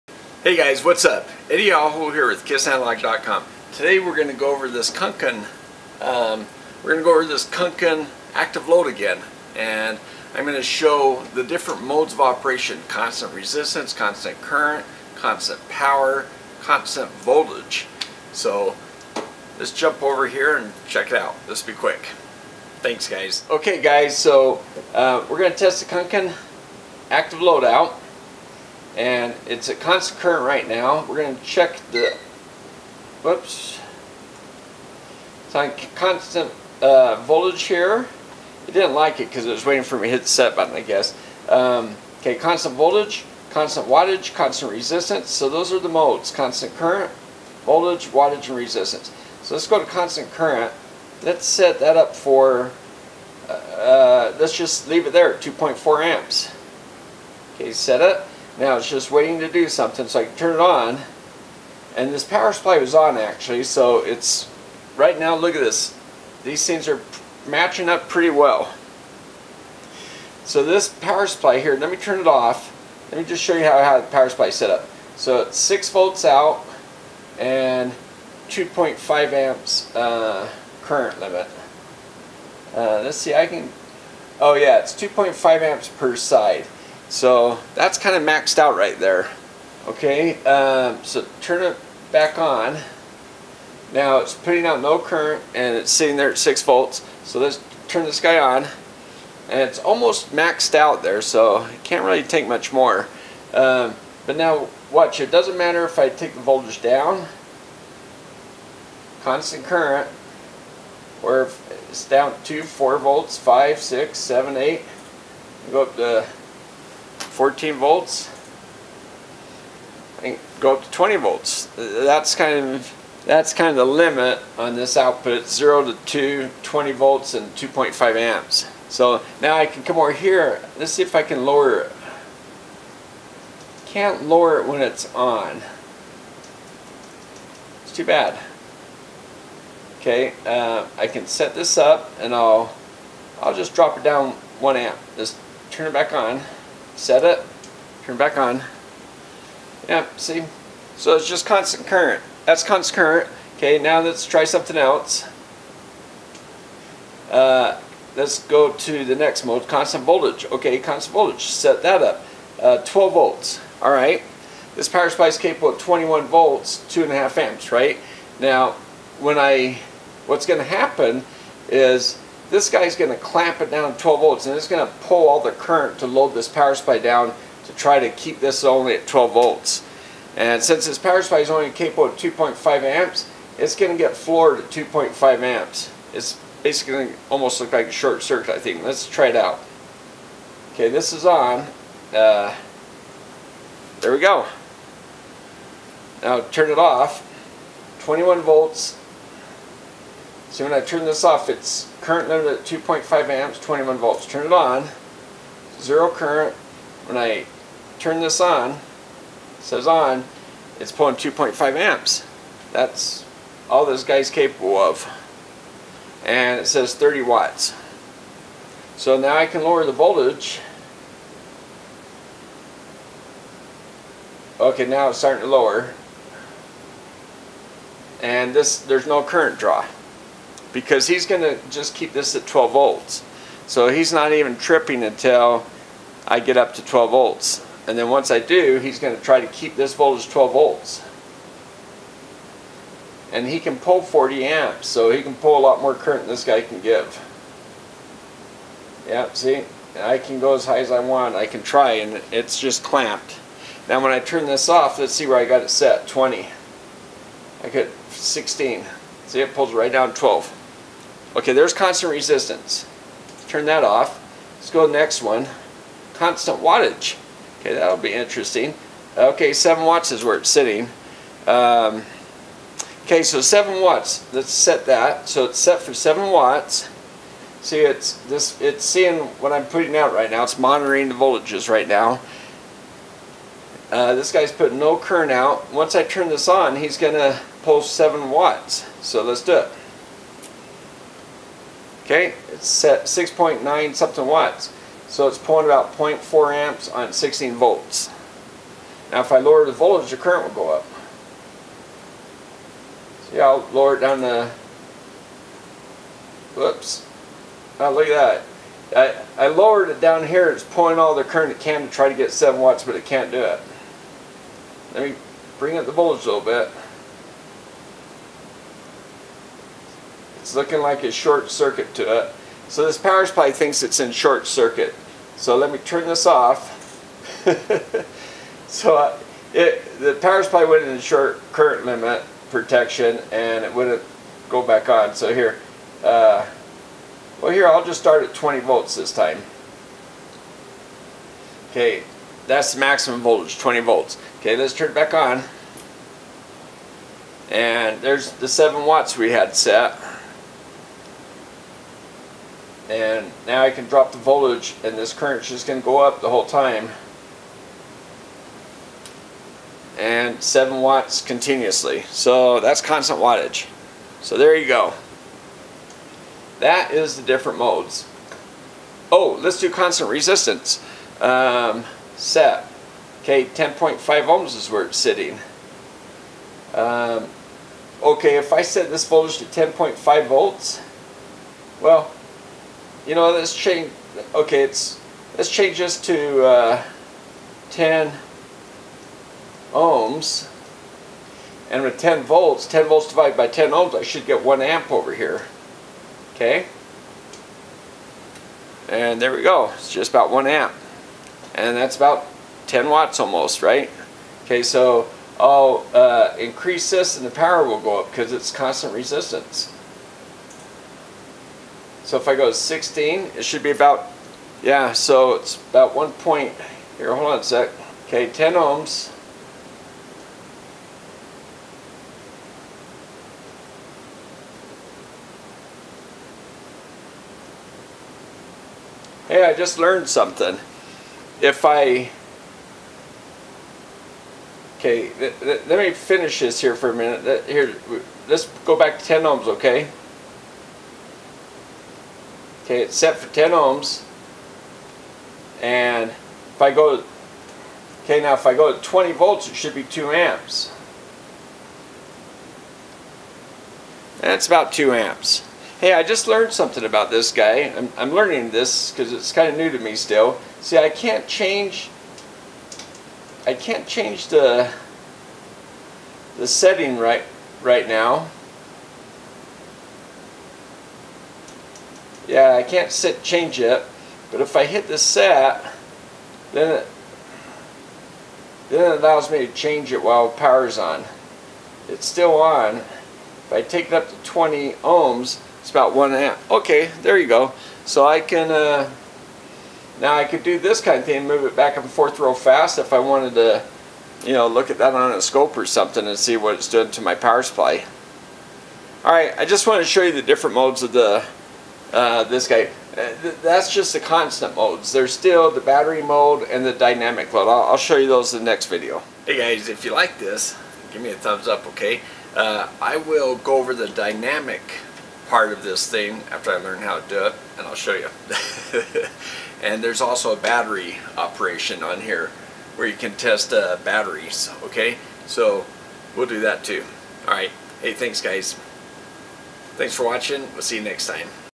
Active Load #2 Kunkin modes of operation
How to use an Active Load.  In part 2 I’ll cover the different modes of operation, i.e., constant current, constant voltage, constant power, and constant resistance using the Kunkin KP184 which runs about $260 on Amazon.

Kunkin KP184